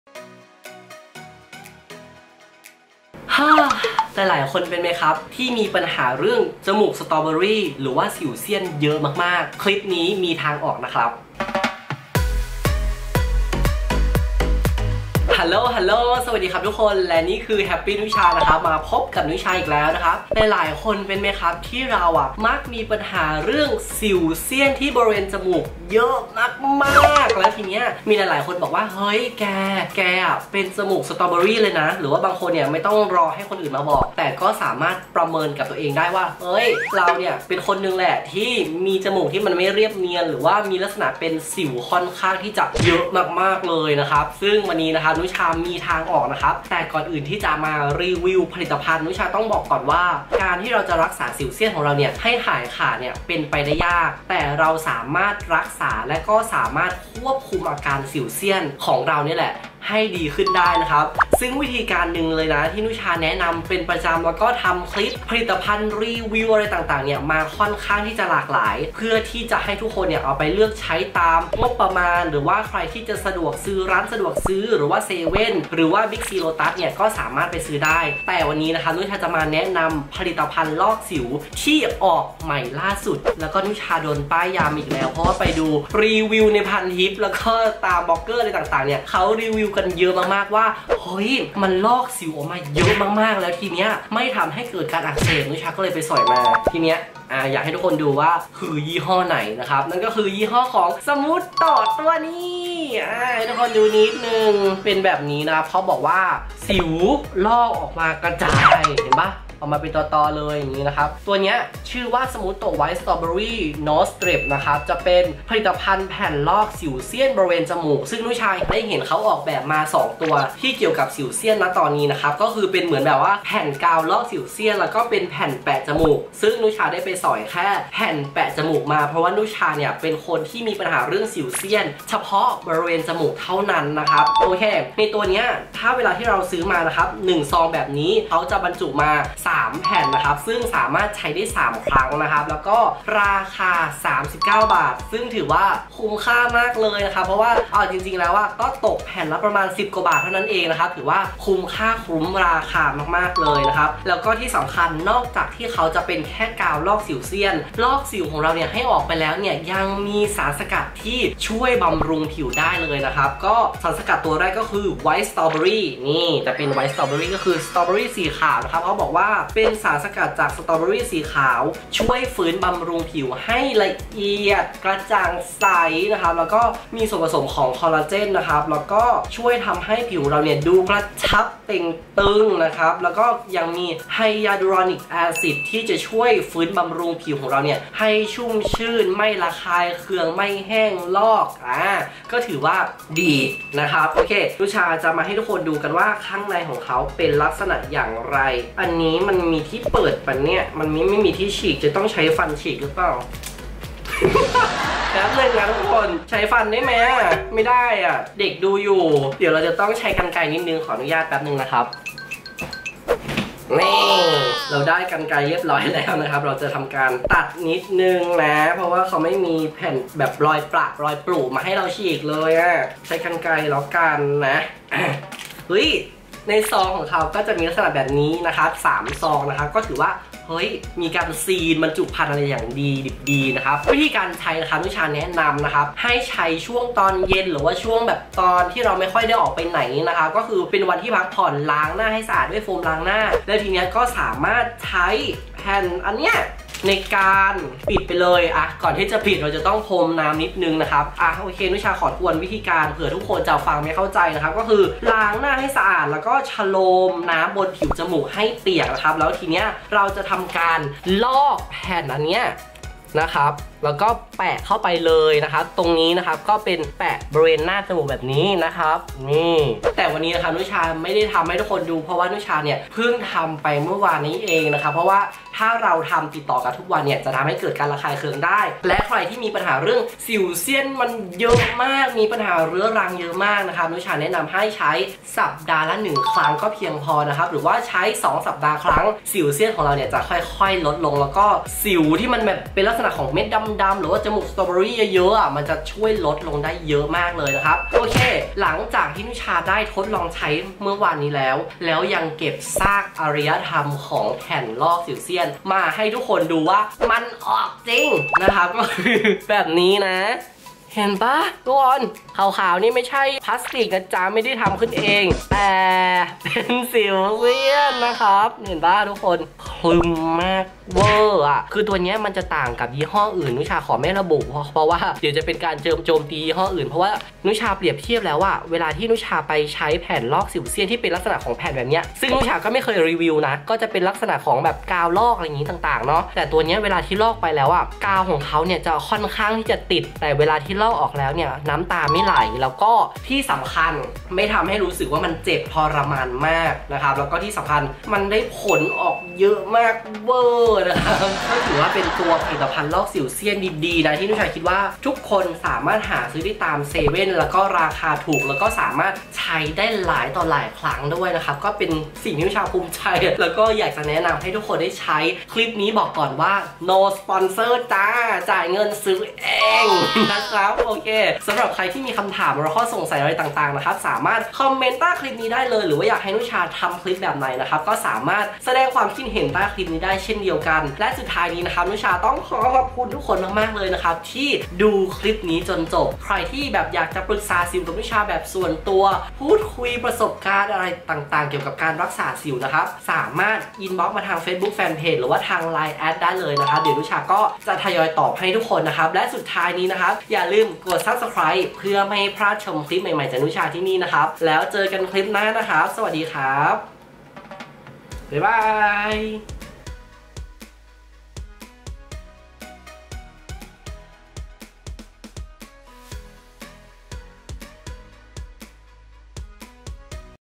[รีวิว 7-11] แผ่นลอกสิวสมูทโตะ Smooto กระชากสิวเสี้ยน ออกมาเป็นตอ !! | นุชา HAPPY NUCHA
[รีวิว 7 11] แผ่นลอกสิวสมูทโตะ Smooto กระชากสิวเสี้ยน ออกมาเป็นตอ !!
♥คุยกับนุชาทางไลน์ Line ปรึกษาสิวแนะนำผลิตภัณฑ์รักษาสิว
Line : @happynucha (มี@ข้างหน้า)
♥ครีมรักษาสิวของนุชา Oranucha Skin
ครีมรักษาสิวสเตียรอยด์ ครีมรักษาสิวเรื้อรัง